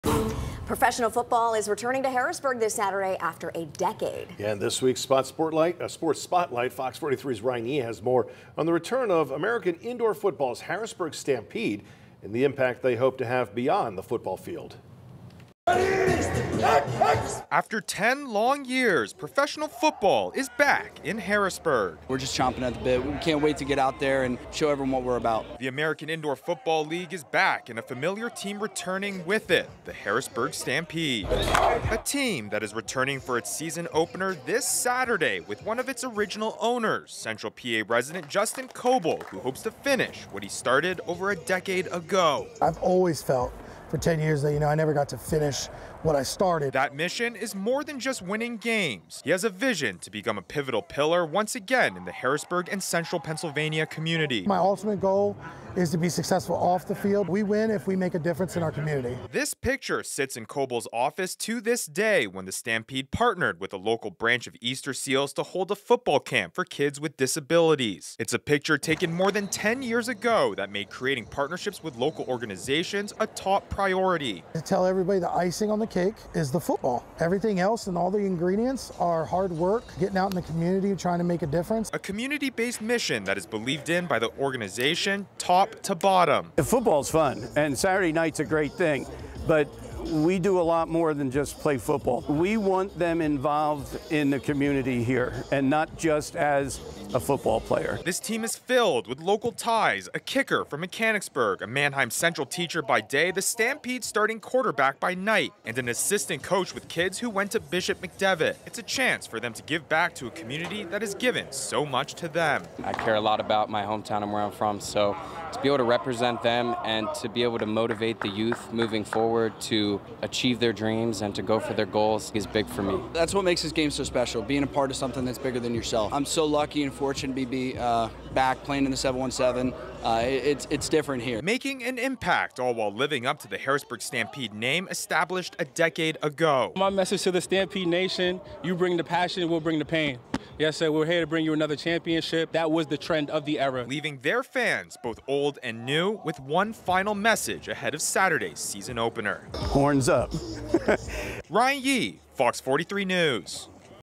Professional football team returns to Harrisburg, aims to make impact on and off the field
Professional football is returning to Harrisburg with American Indoor Football (AIF) set to relaunch and kick off its 2024 season this weekend.

After 10 long years, the Harrisburg Stampede is back with its season opener against the Cedar Rapids River Kings on Saturday at 7 p.m. at the Pennsylvania Farm Show Complex.

"We’re just chomping off the bit," said Caleb Walton, the quarterback for the Harrisburg Stampede. "We can’t wait to get out there and show everyone what we’re about."

The team is returning under the helm of some familiar faces. Justin Coble, a central Pennsylvania resident who owned the team in 2012 before selling his ownership share to then co-owner and former NFL player Marques Colston, is back as team owner.